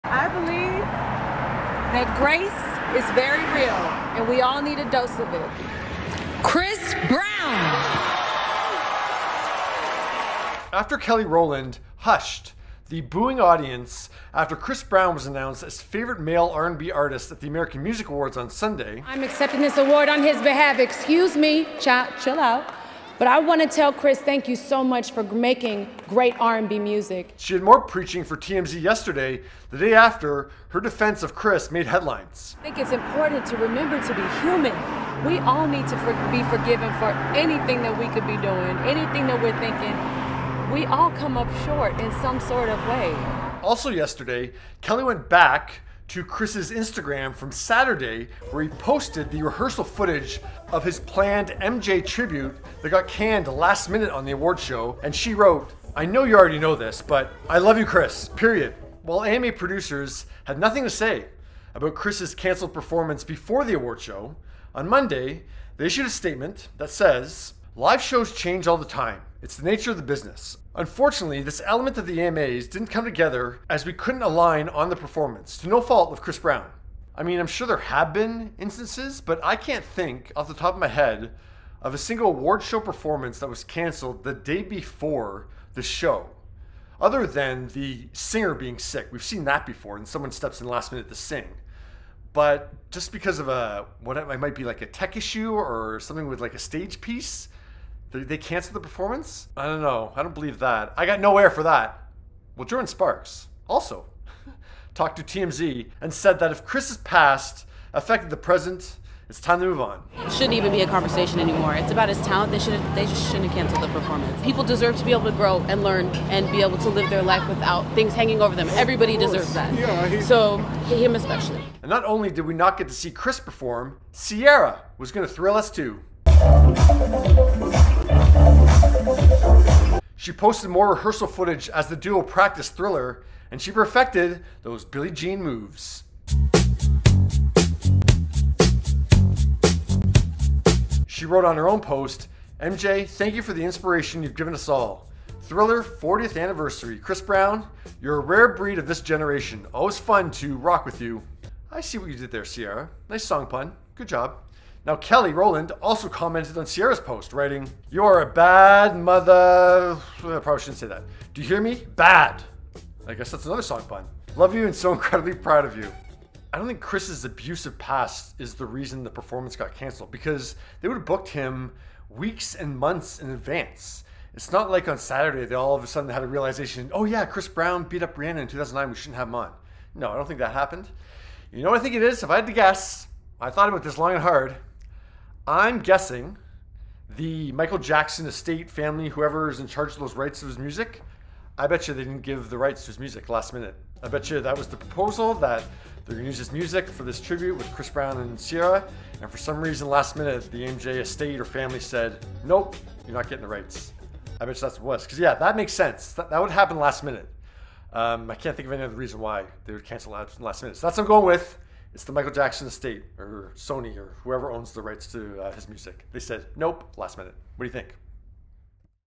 Kelly Rowland, Jordin Sparks Protest Chris Brown, Ciara's Cancelled MJ Tribute
Graeme O'Neil takes a look at the new footage of Ciara joining Chris Brown in rehearsal for a planned Michael Jackson tribute at the 2022 American Music Awards that was canned last minute, and how Kelly Rowland, and now Jordin Sparks are showing their distain for the decision.